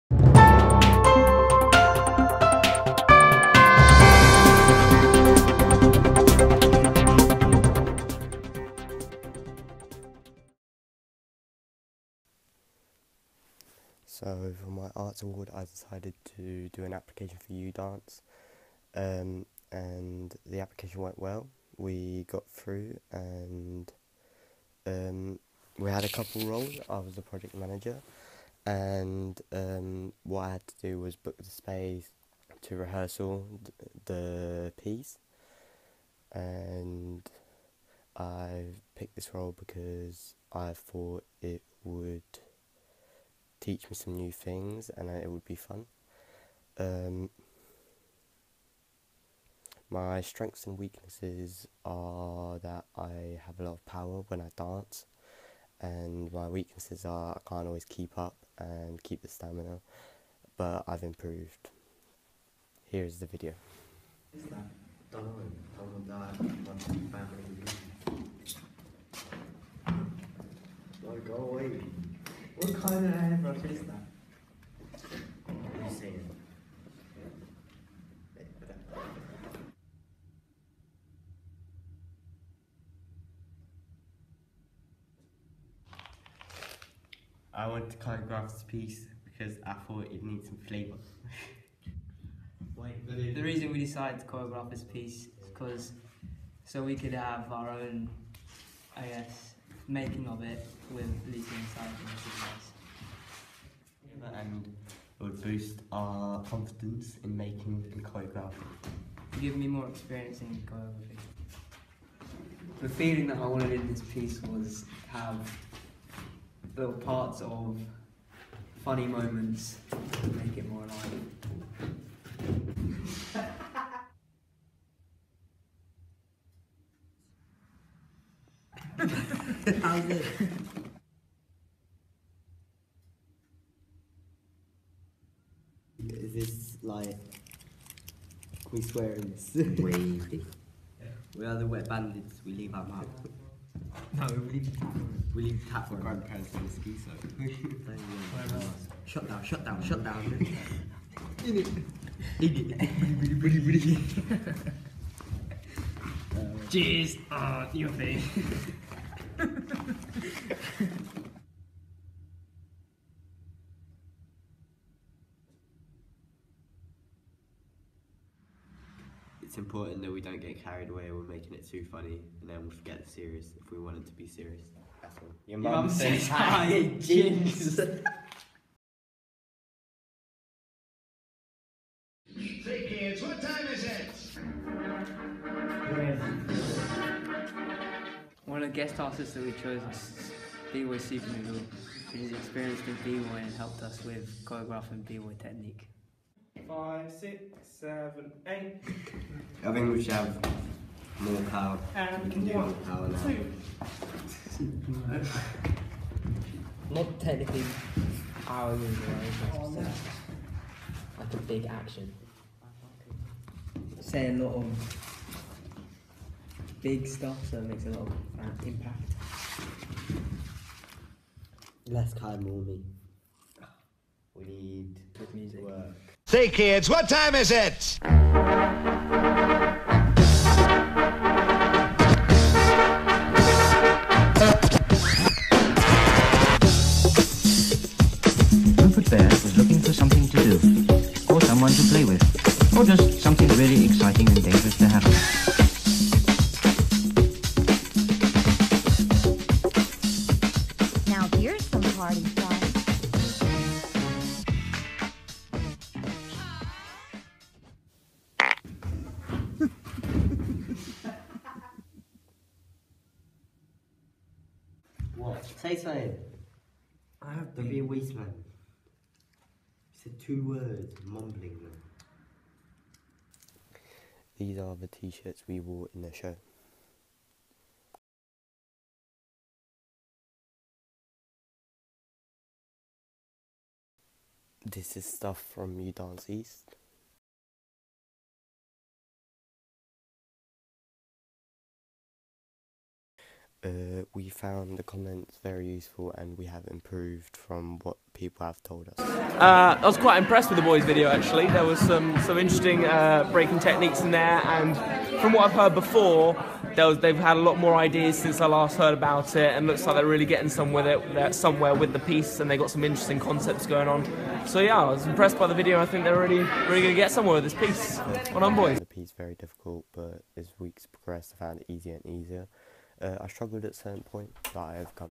Szymon Arts Awards Unit 1, Part A & B
Arts Award Silver, Unit 1 part A and B, by Szymon Zagorski. Arts Challenge to put together an application for U.Dance